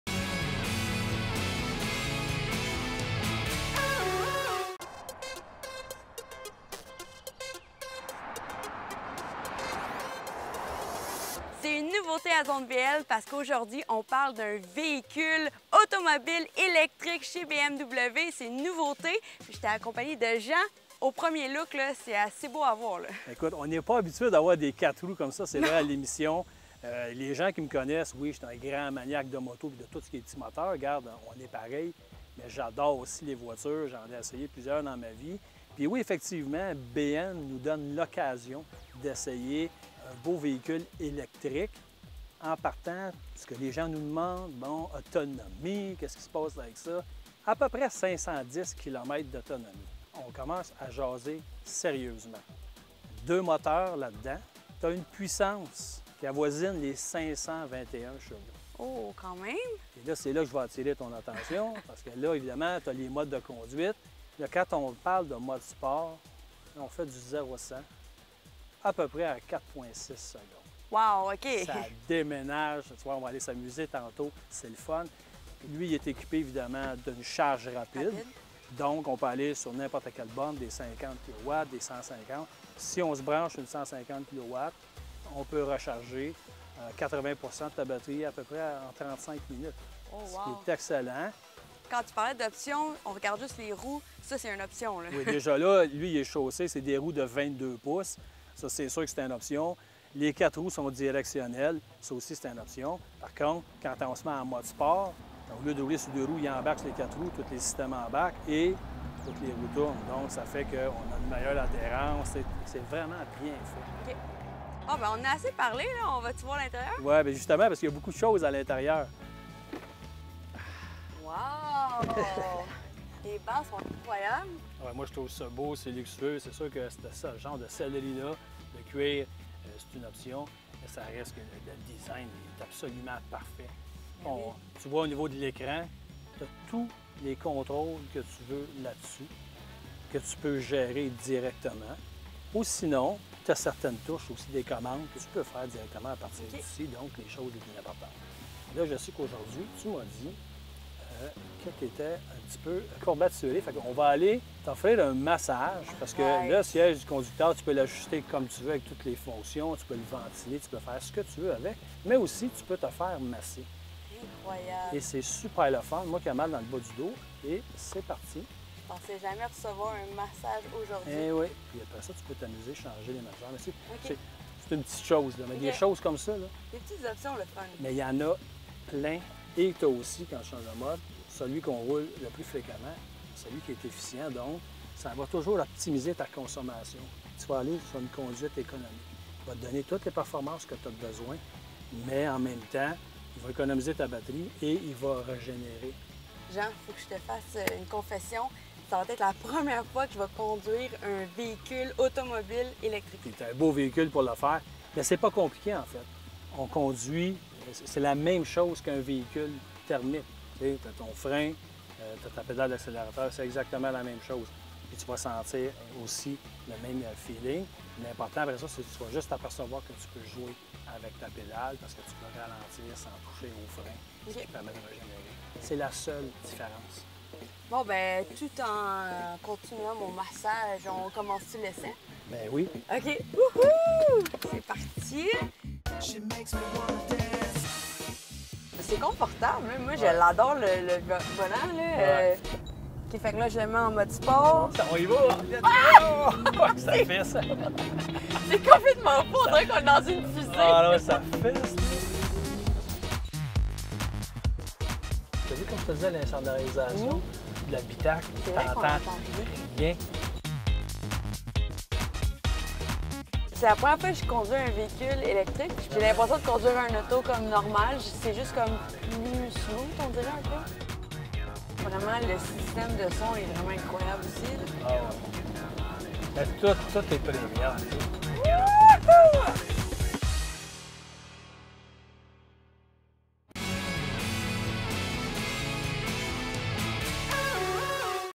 Véhicule électrique BMW iX
Jean Paré présente à Cynthia Gauthier le véhicule électrique BMW iX.